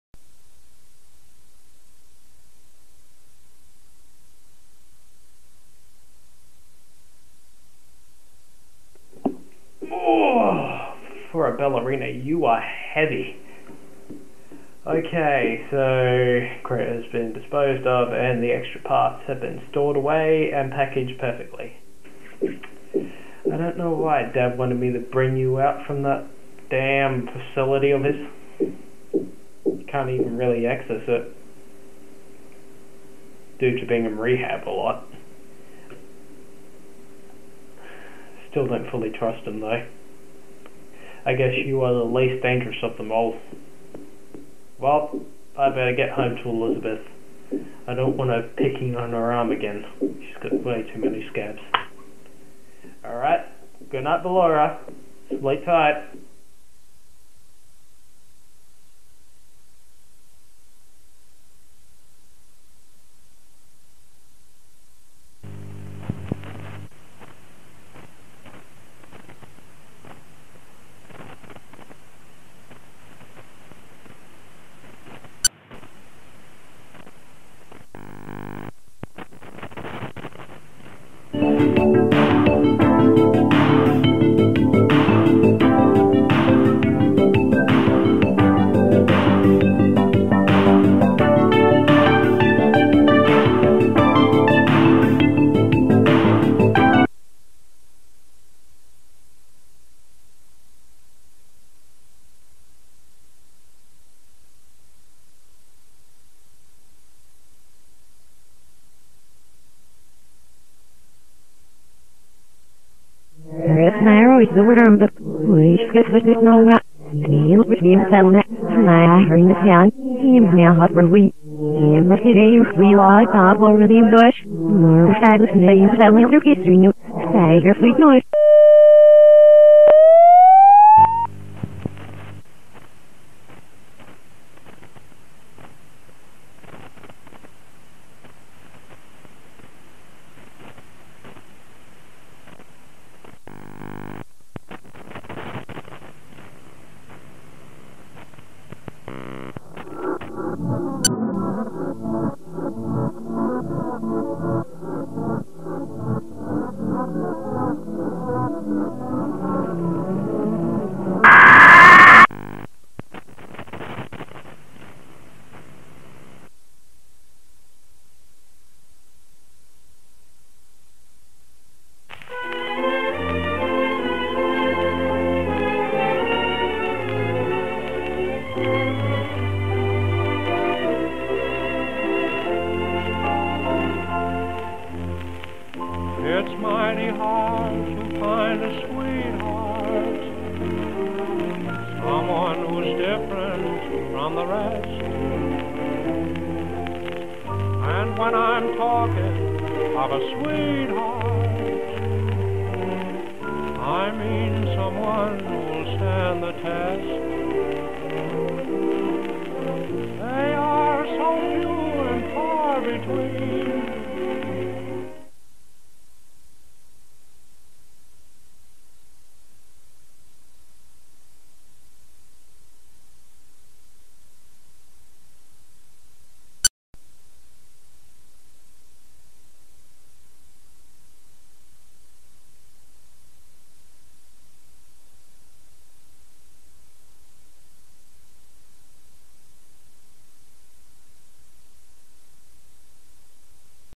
[Fazbear Ent Lost Tapes] clarascall.mp4
This video was recorded during the closure of Circus baby Entertainment Rentals, which closed due to financing problems, but has been the target of many Conspiracy theorist's .

[Tape and Series Inspired by Squimpus Mcgrimpus, But is not afflicted with his series